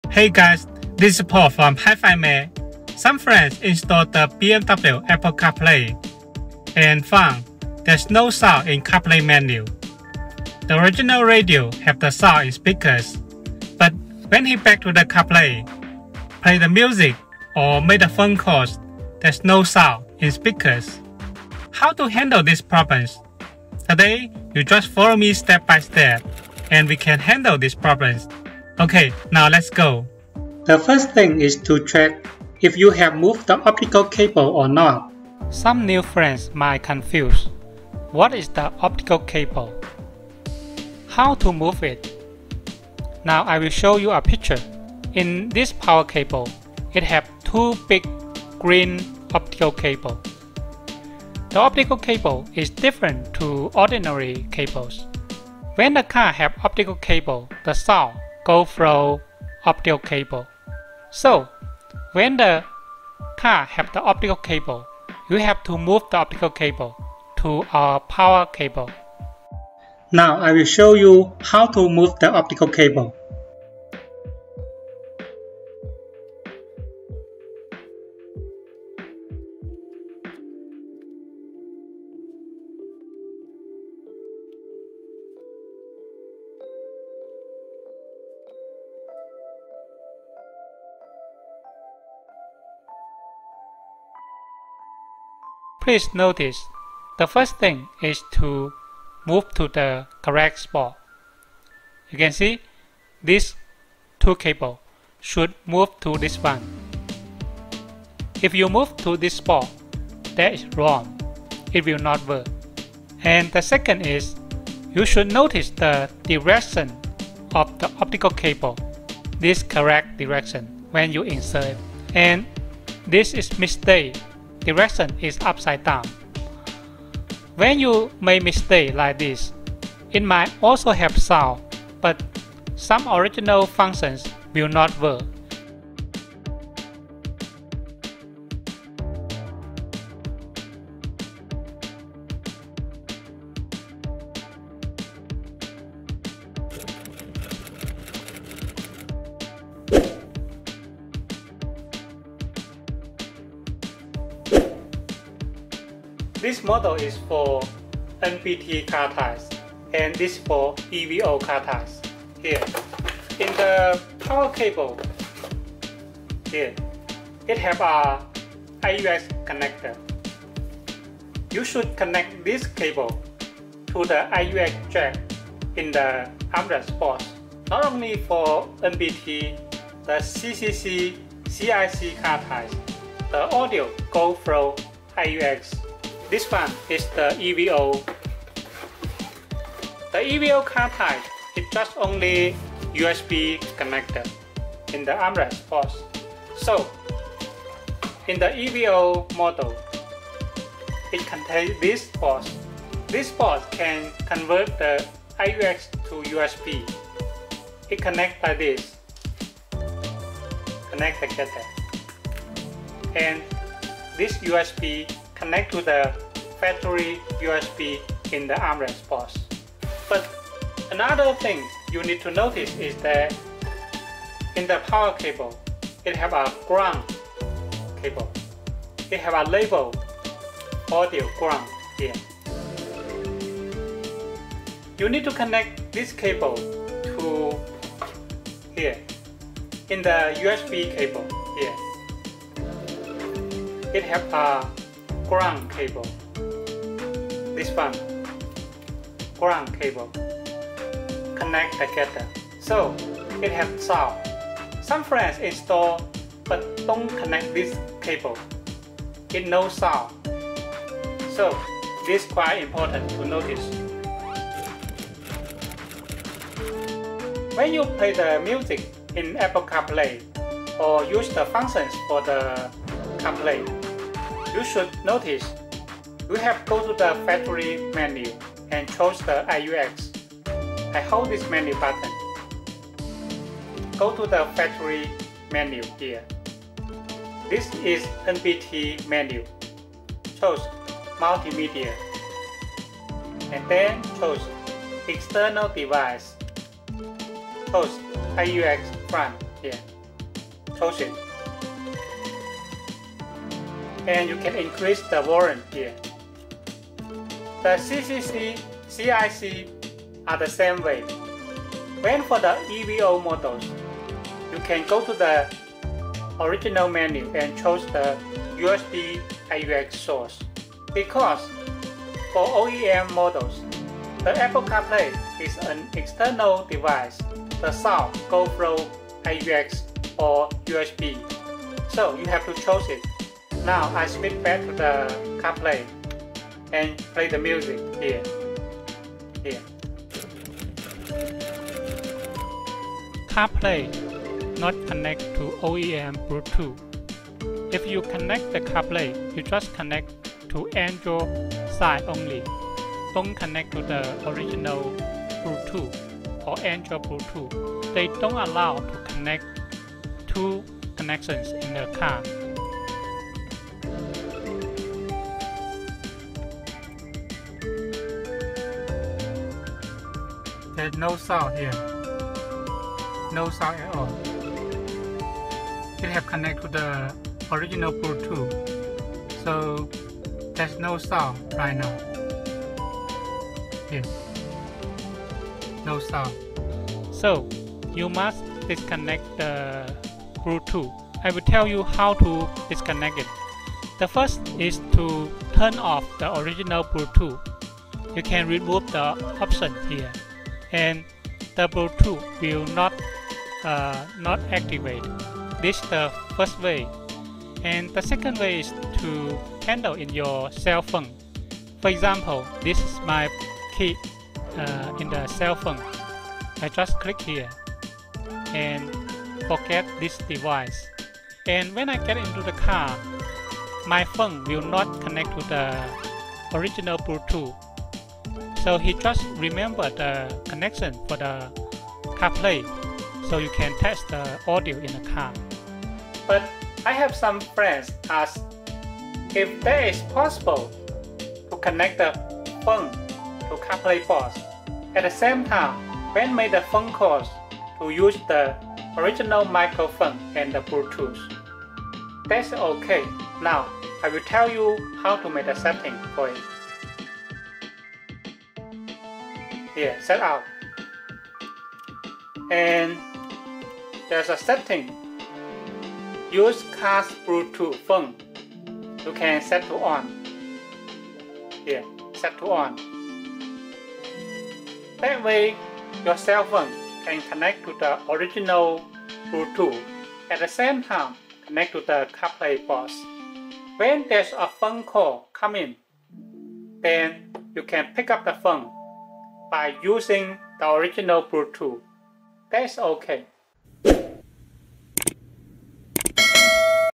BMW Apple Carplay Android Auto NO Sound Fixed | BMW wireless Carplay without Audio trouble shooting!
Aftermarket BMW Apple Carplay Android Auto NO SOUND Fixed! Step by step guide to handle the BMW Apple Carplay NO Audio problem!